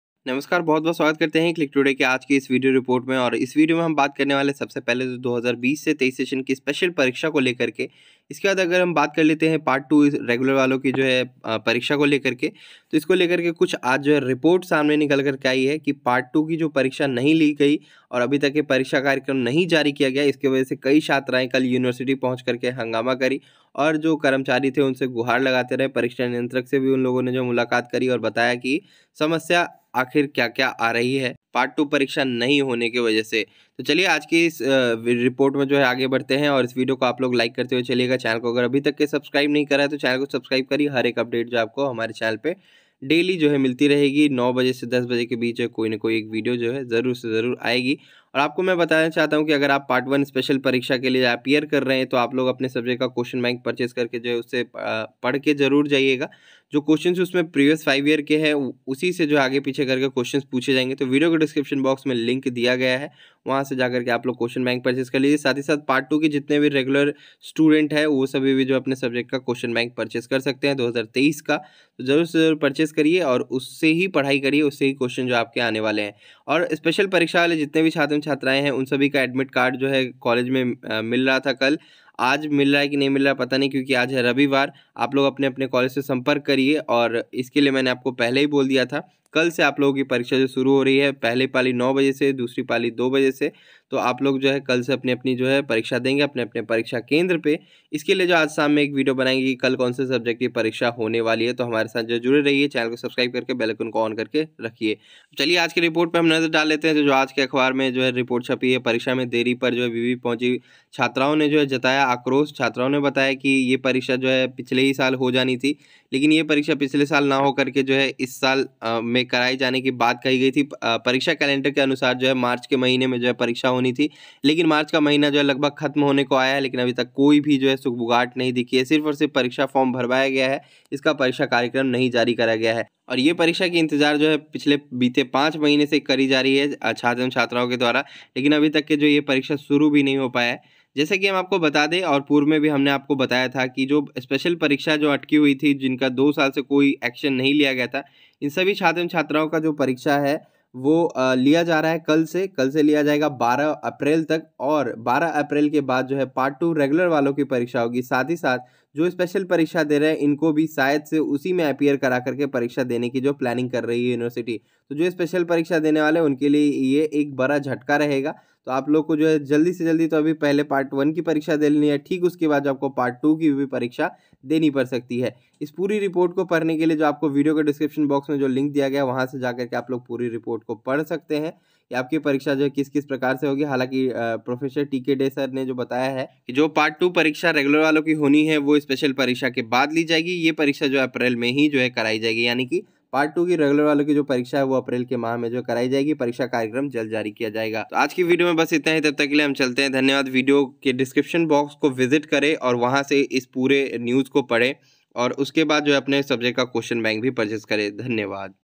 brabu news: part 2 exam date 2020-23,part 1 special exam 2020-23,आज की ताजा खबरें, brabu university
In today's video, we will be discussing some of the recent developments that have taken place in Bihar University.

Firstly, the university has announced the dates for the upcoming semester exams. According to the latest notification, the exams will begin in the second week of April and continue until the end of May. This will give students enough time to prepare for their exams and revise their syllabus.

Secondly, Bihar University has introduced a new course in collaboration with a leading industry player. The course is designed to provide students with hands-on experience in the field of data analytics and artificial intelligence. This initiative is aimed at bridging the gap between academia and industry and preparing students for the job market.

Thirdly, the university has also launched a new initiative to promote research and innovation among students and faculty members. The initiative includes a grant program to support innovative research projects and a series of workshops and seminars to promote research culture on campus.

Finally, Bihar University has also taken several measures to improve the infrastructure and facilities on campus. The university has renovated several buildings and classrooms, installed new equipment in laboratories, and upgraded the library with new books and journals.

So, these are some of the recent developments that have taken place in Bihar University.

brabu university part 2 exam ba b.com b.sc
brabu ba part 2 ba exam news